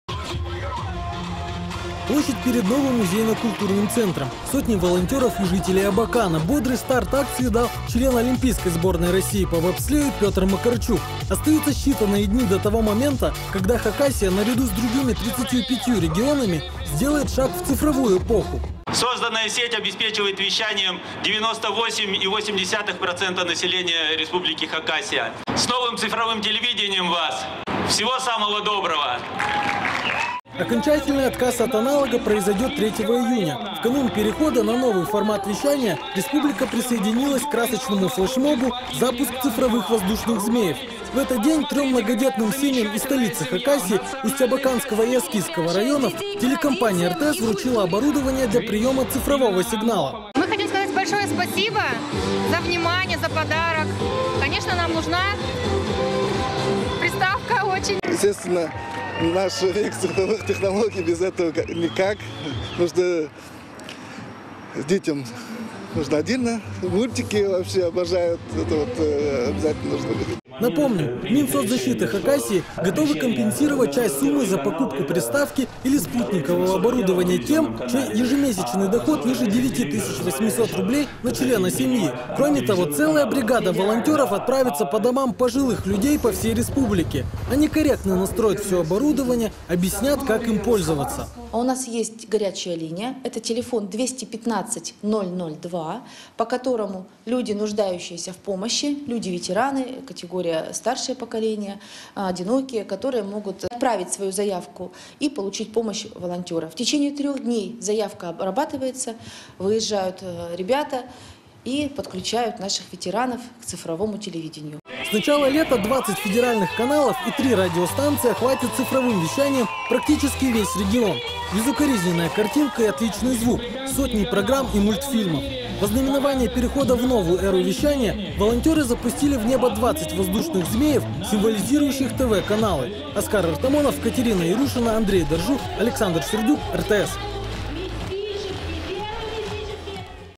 Полет "цифрового змея": оригинальный флешмоб собрал сотни абаканцев
Хакасию накроет "цифровая волна". Напомним, с 3 июня в республике прекращается аналоговое вещание, а ему на смену придут 20 федеральных телеканалов и три радиостанции в формате высокой четкости изображения и звука. В преддверии важной даты в столице Хакасии состоялась красочная акция "Запуск цифровых воздушных змеев".